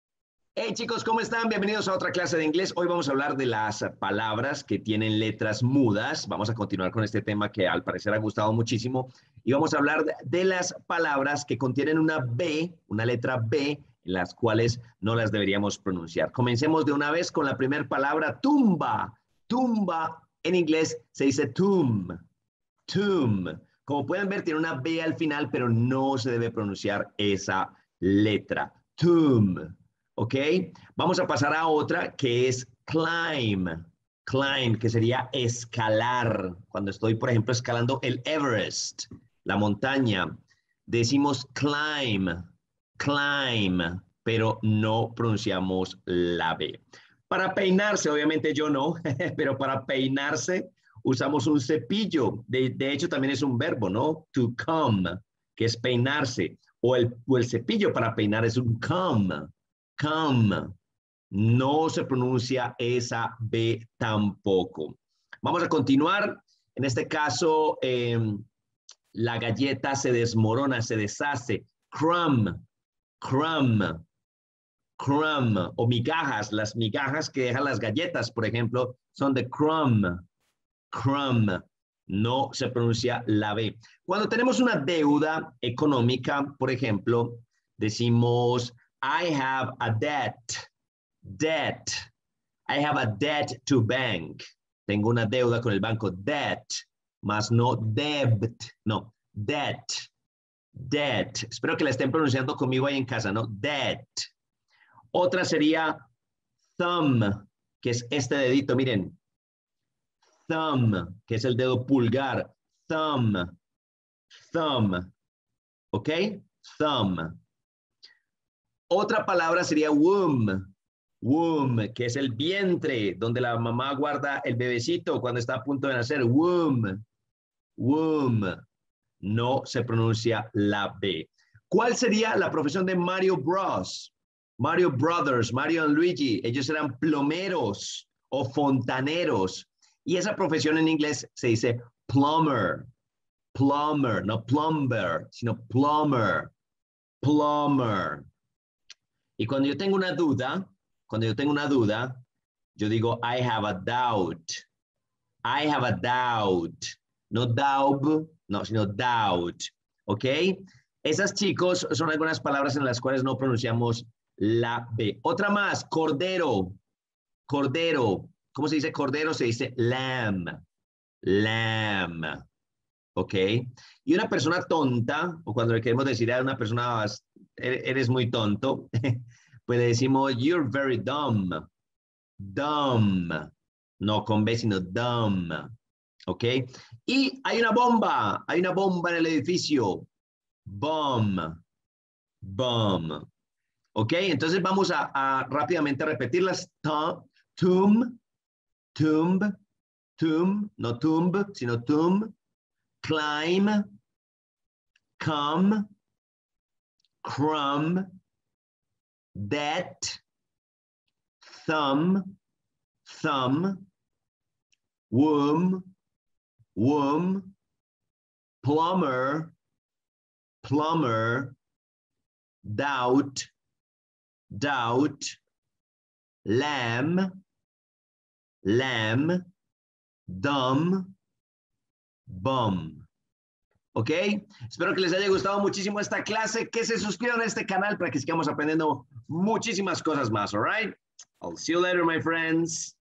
NO pronuncies la B en estas palabras de INGLÉS
Hoy vamos a hablar de Silent Letters y hablaremos del a letra B la cual no se debe pronunciar en estas palabras.